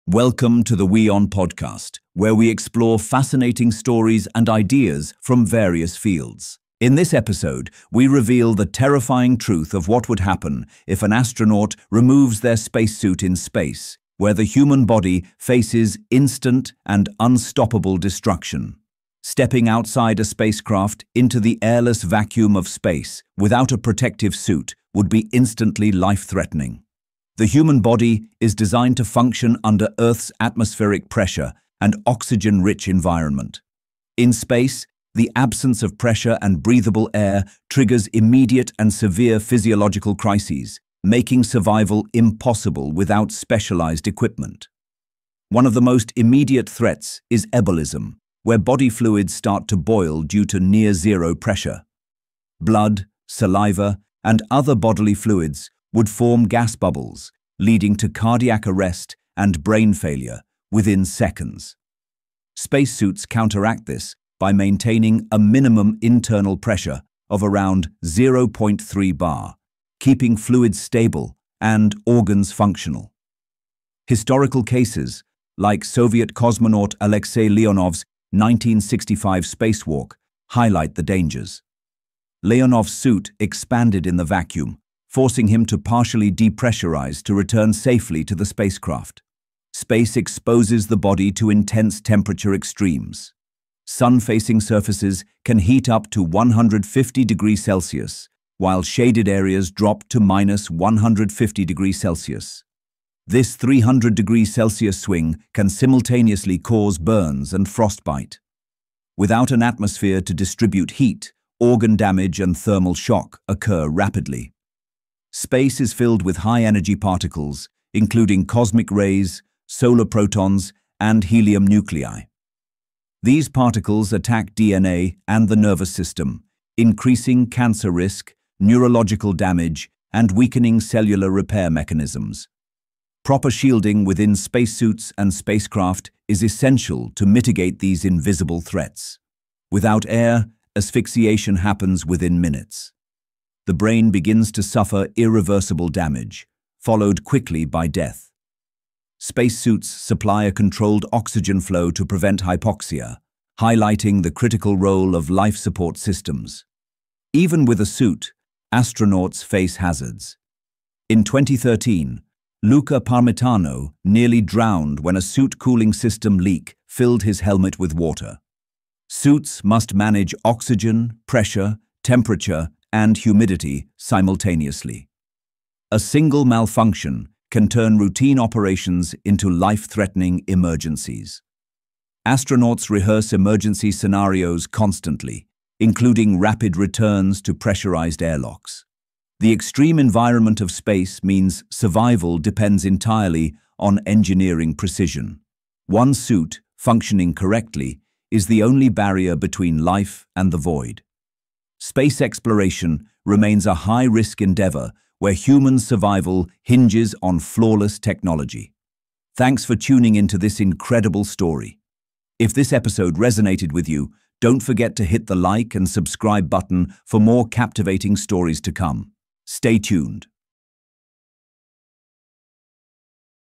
What Happens Without a Spacesuit in Space? | WION Podcast
In this episode, we uncover the terrifying truth of what happens if astronauts take off their spacesuit in the vacuum of space. Without atmospheric pressure or oxygen, the human body suffers instantly from ebullism, where fluids start to boil, leading to organ failure and death within seconds. Extreme temperature swings of +150°C to -150°C can burn and freeze at the same time, while cosmic radiation attacks DNA and the nervous system. Asphyxiation occurs within minutes, proving why spacesuits are vital to survival. Even with suits, astronauts face risks, as shown in past emergencies, highlighting how one piece of technology is the only shield between life and the deadly void.